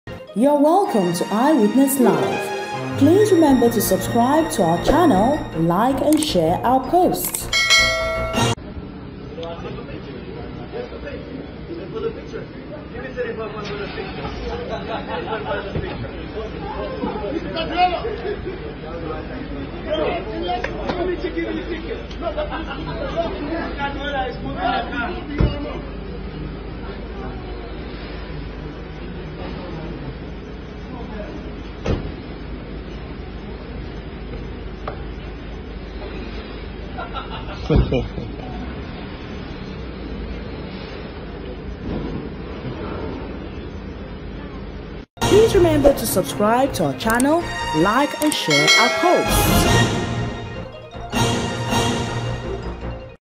Manchester City Coach Pep Guardiola: Pay for the Photos?! 📸💰
Manchester City Coach Pep Guardiola: Pay for the Photos?! 📸💰 | Hilarious Encounter with Transport Official 😄🚗
Pep Guardiola gets ticket for Parking on Double Yellow